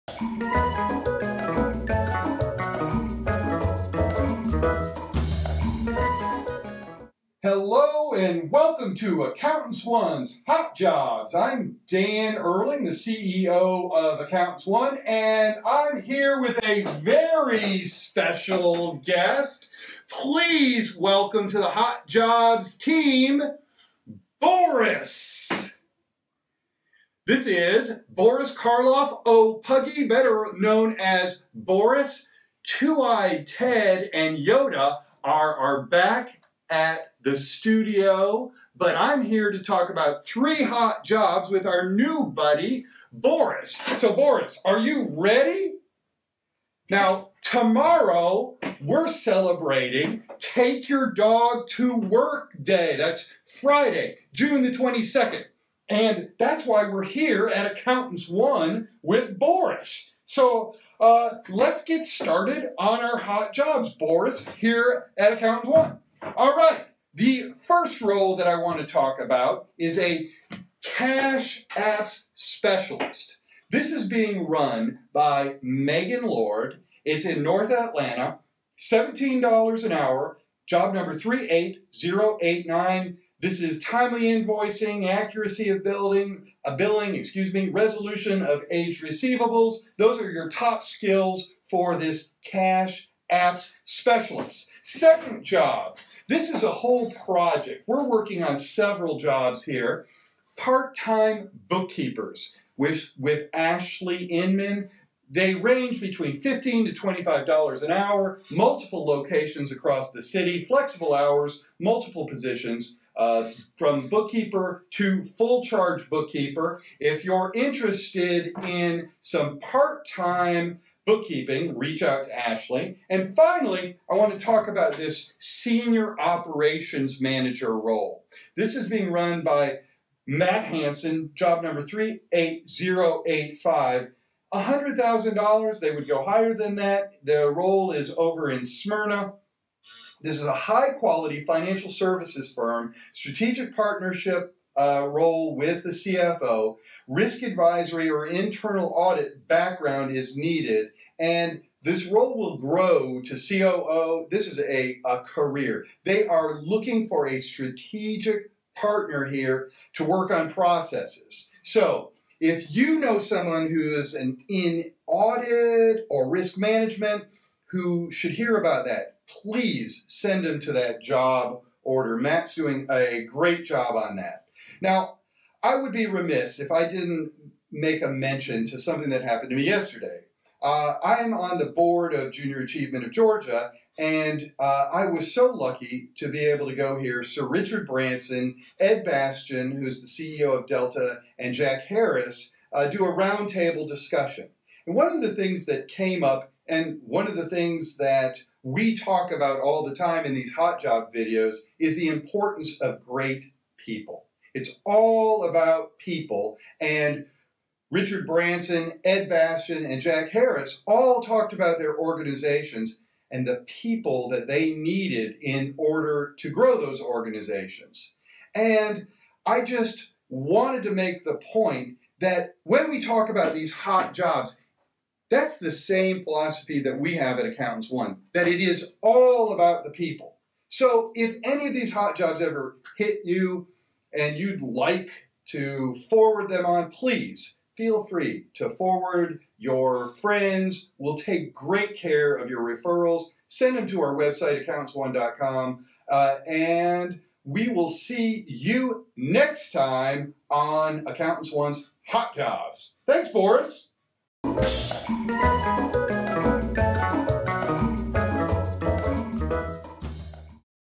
Accountants One Hot Jobs: Bring your Dog to Work Day Edition - June 22, 2018
Friday, June 22 is Take Your Dog to Work Day!

To celebrate, Dan introduces us to the latest member of the Accountants One Hot Job team -- Boris. We will collect and post them next week.

Dan also gives us his main takeaway from attending a Junior Achievement of Georgia event with Sir Richard Branson, Ed Bastian, and Jack Harris this week.

These great leaders reminded him that it really is ALL ABOUT THE PEOPLE!

HOT JOBS:
Hot Job 1: Cash Apps Specialist
Hot Job 2: Part-time Bookkeeper (several positions)
Hot Job 3: Senior Operations Manager